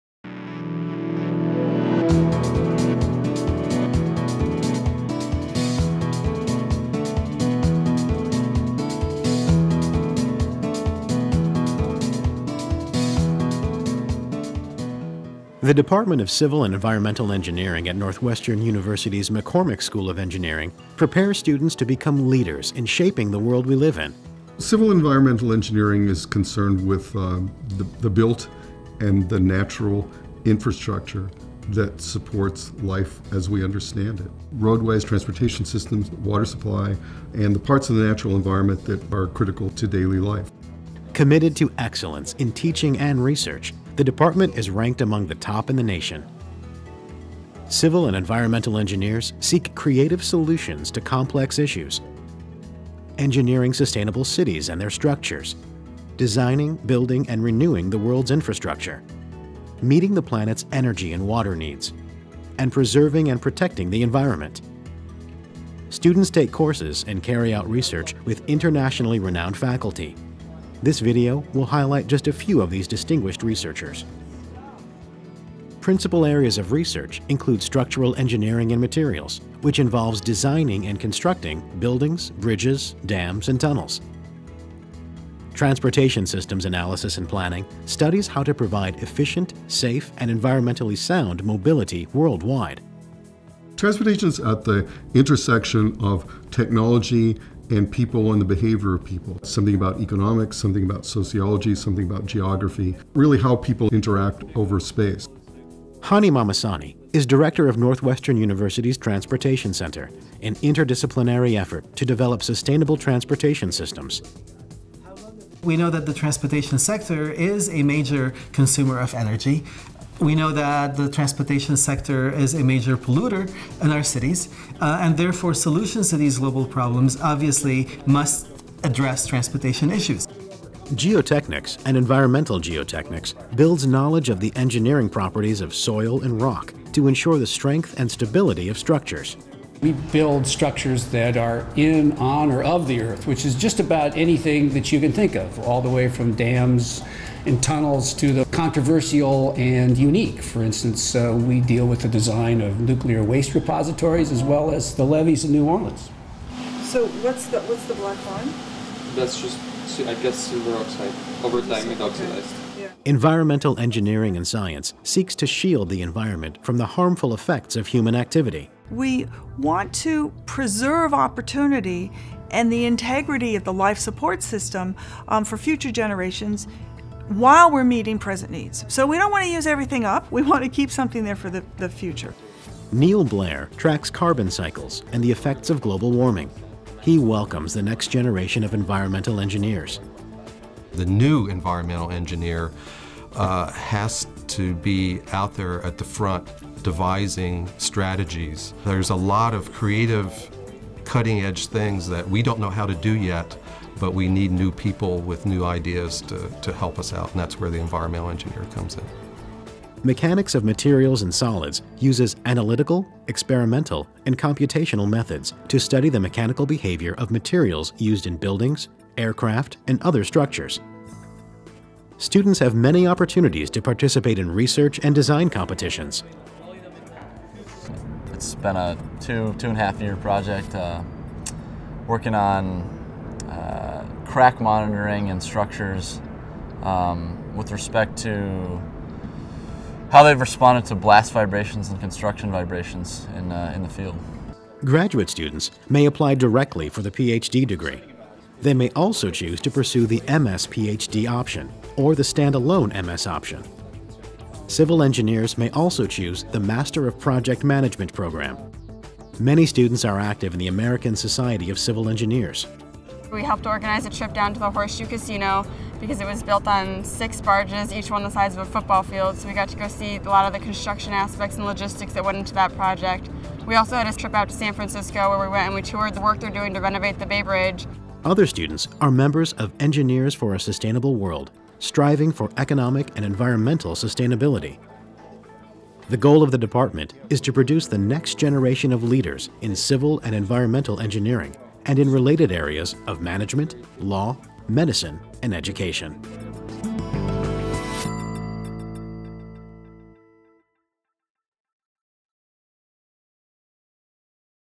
Civil and Environmental Engineering
An overview of the Department of Civil and Environmental Engineering featuring interviews with professors Joseph Schofer, Hani Mahmassani, Charles Dowding, Kimberly Gray, and Neal Blair ____